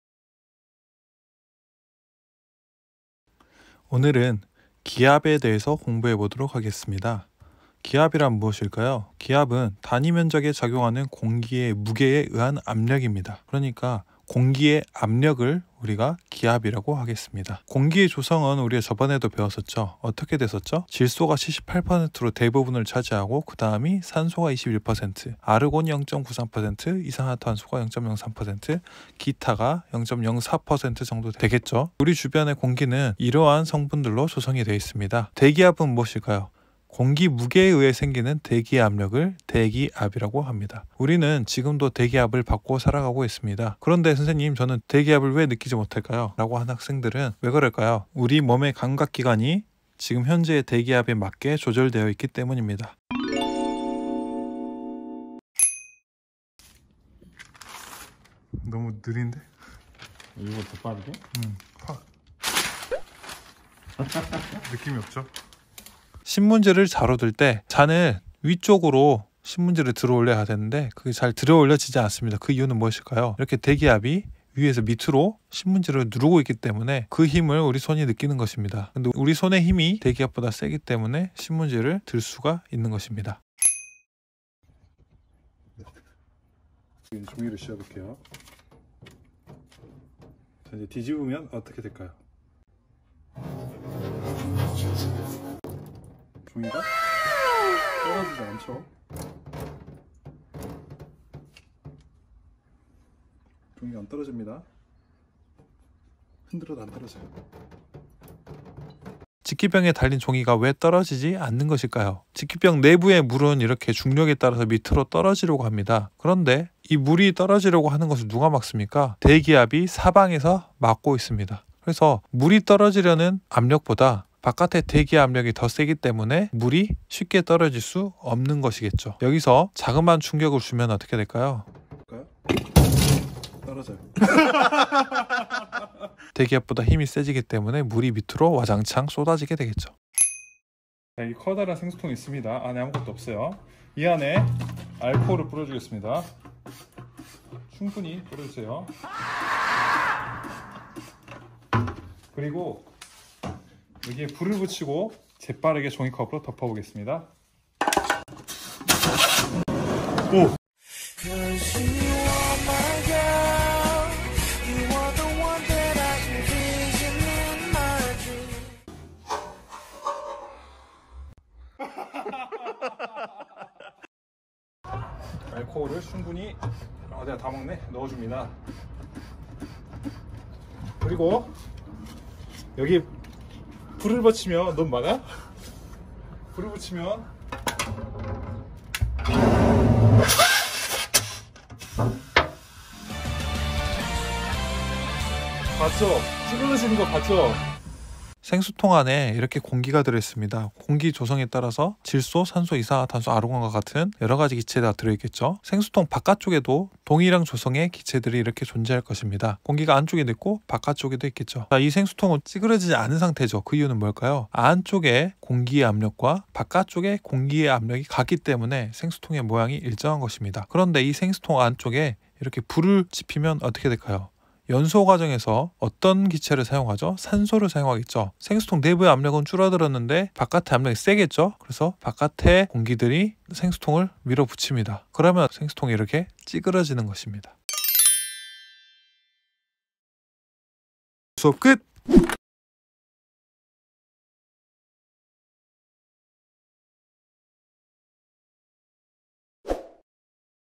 [KING] 중 3 과학 16강 기압
Ⅱ. 기권과 날씨
3. 기압과 날씨
01. 기압 차이로 부는 바람